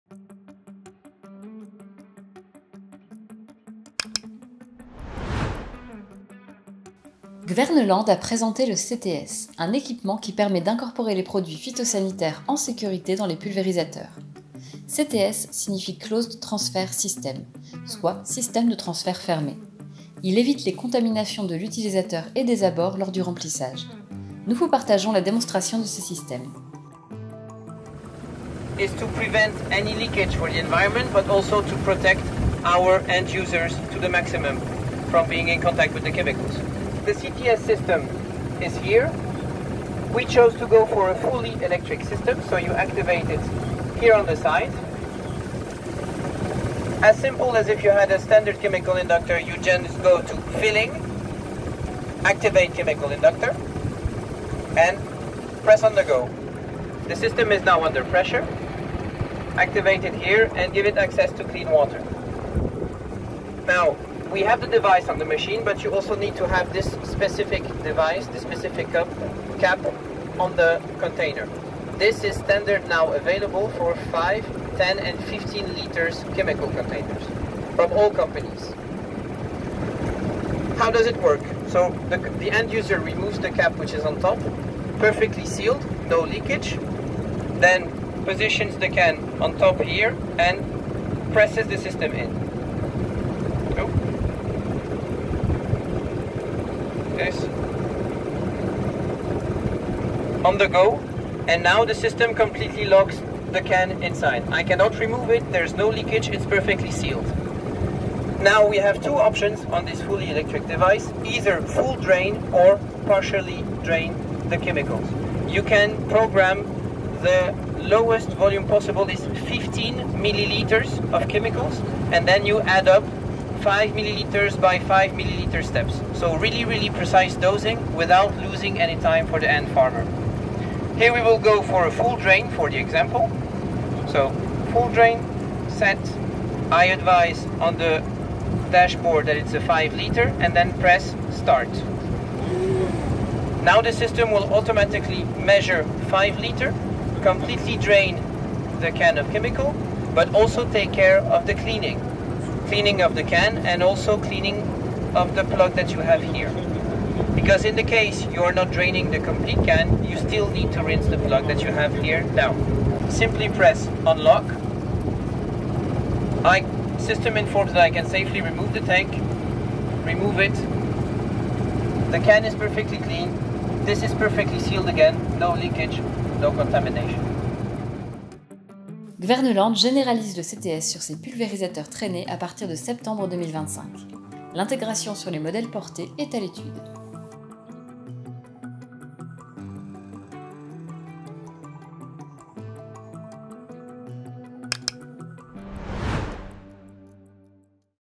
Avec le CTS, Kverneland remplit les pulvérisateurs en sécurité
Kverneland a présenté le CTS, un équipement qui permet d’incorporer les produits phytosanitaires en
sécurité dans les pulvérisateurs. CTS signifie « Closed Transfer System », soit « système de transfert
fermé ». Il évite les contaminations de l’utilisateur et des abords lors du remplissage. Nous vous
partageons la démonstration de ce système.